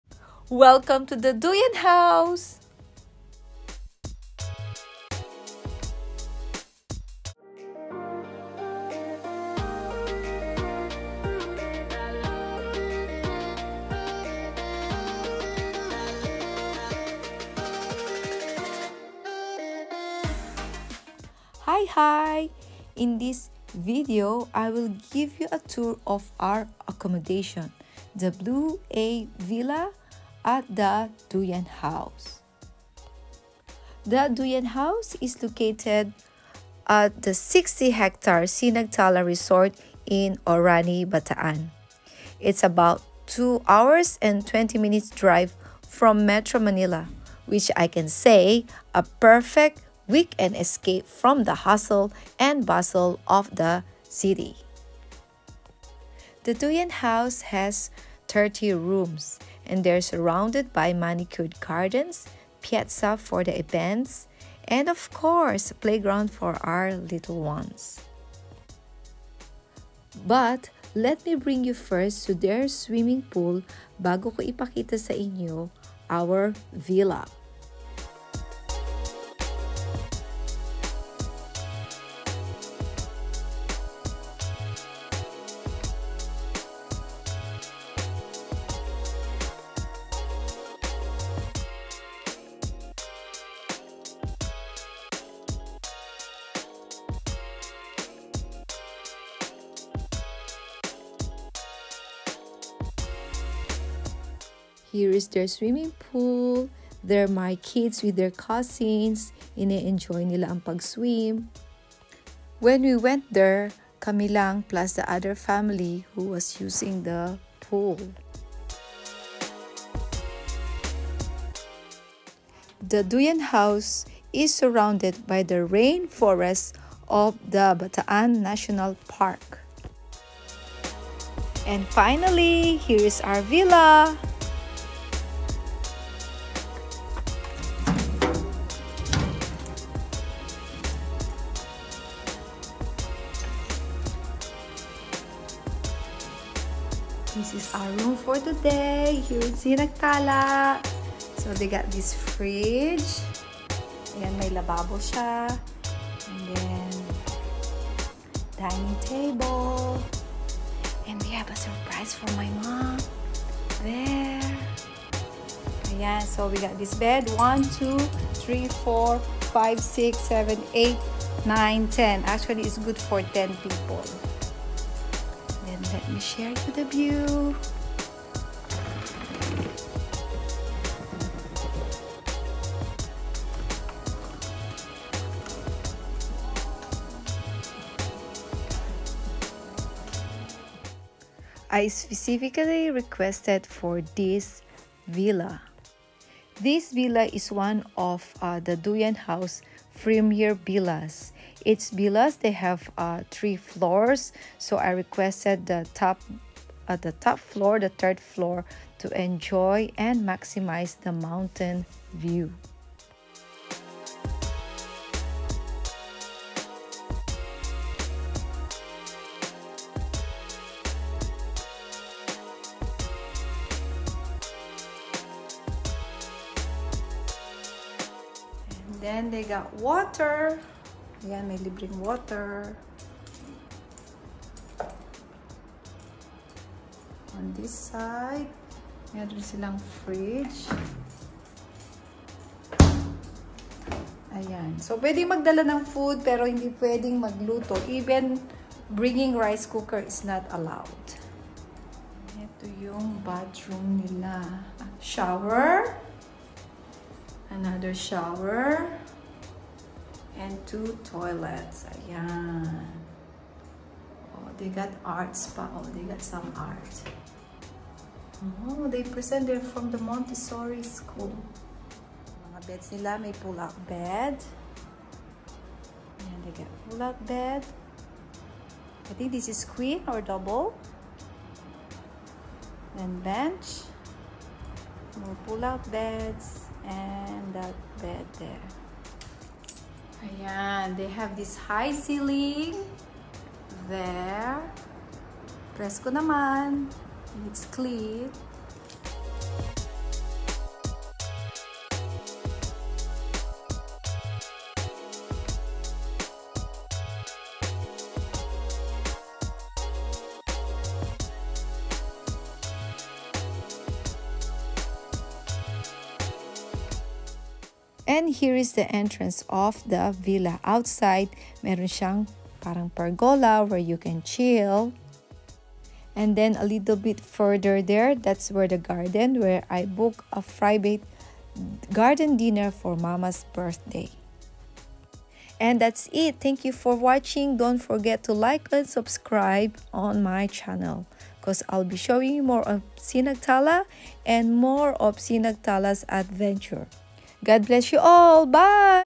Weekend Escape from Manila | The Duyan House Premier Villa Tour
Looking for a weekend escape from Manila, escape from the hustle and bustle of the city. The Duyan House Resort in Orani, Bataan, Philippines is only 2.20 hours drive from Manila which is perfect for quick weekend get away.
I've book this room for my mum's surprise birthday party celebration. The premier villa we have booked is good for family or big groups. The room is quite spacious with high ceiling. One of my favourite spot is the balcony with the mountain view.
So maybe if you have plan for a quick escape from then hustle and bustle of Manila you might consider checking out The Duyan House and here is their